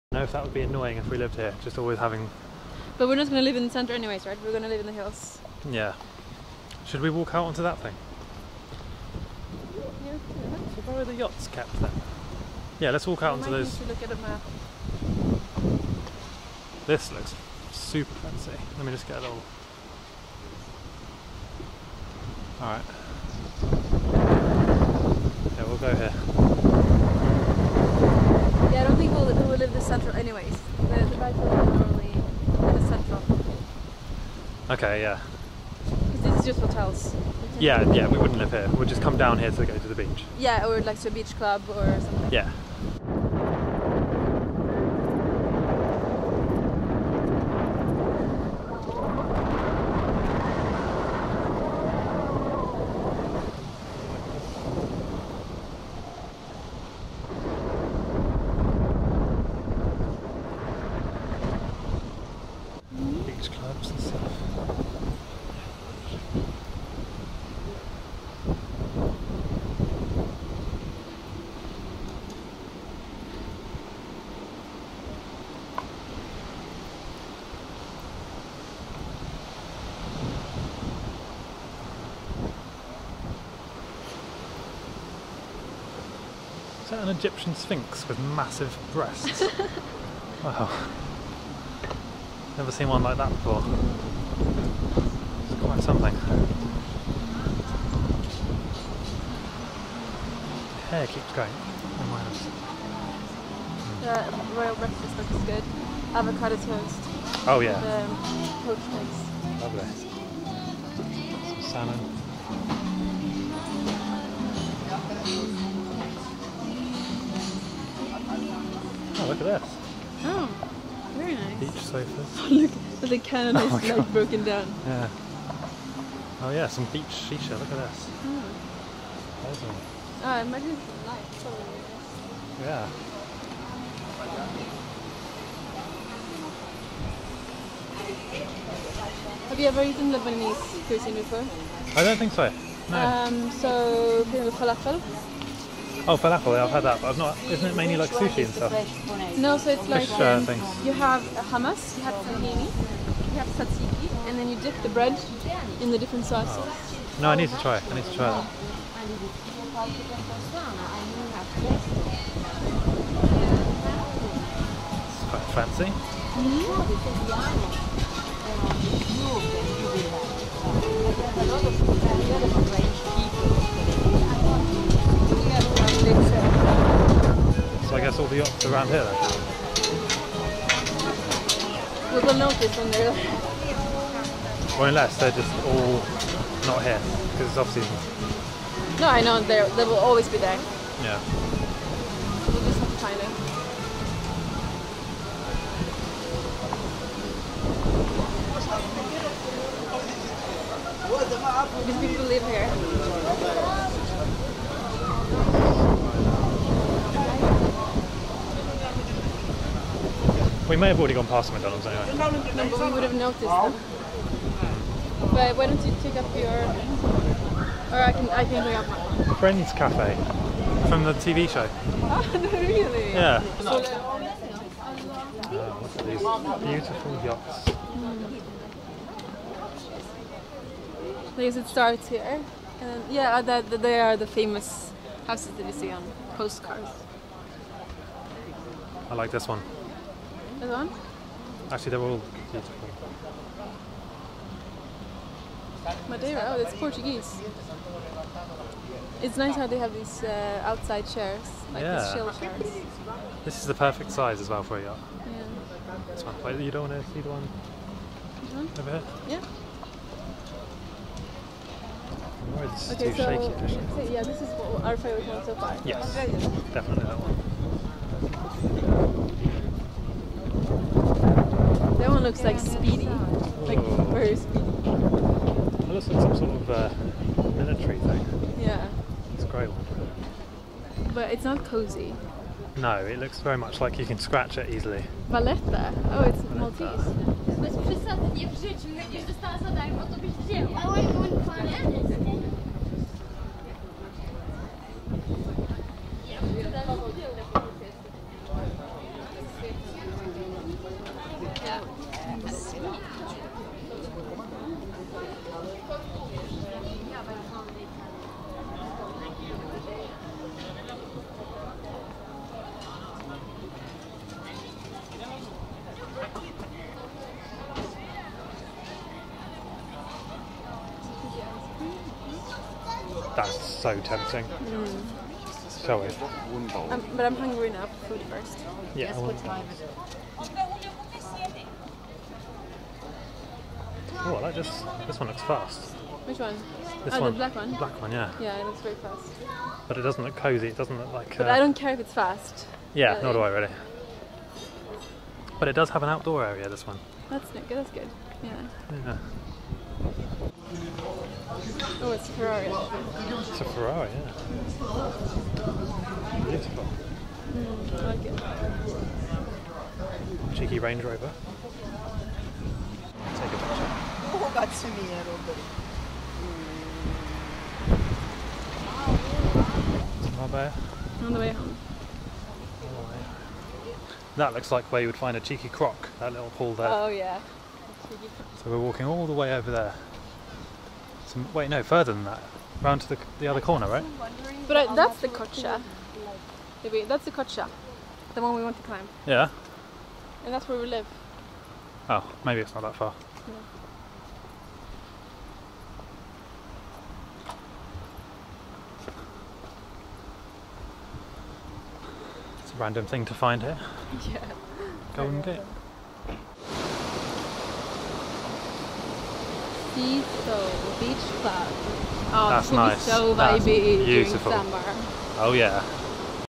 MILLIONAIRES Yachts In Puerto Banus: Marbella Street Walking In November 2019
. MARBELLA, PUERTO BANUS - We walked around the rich area of Spain, Puerto Banus. Many millionaires dock their yachts here, and you can tell the wealth that's floating around this area (See what I did there?). There were so many yachts herein Puerto Banus, I lost count of how many there were. It's a lovely area of Marbella and not too far from the Costa Del Sol.

The Camera I Use

My names Stef and I make travel videos on this channel.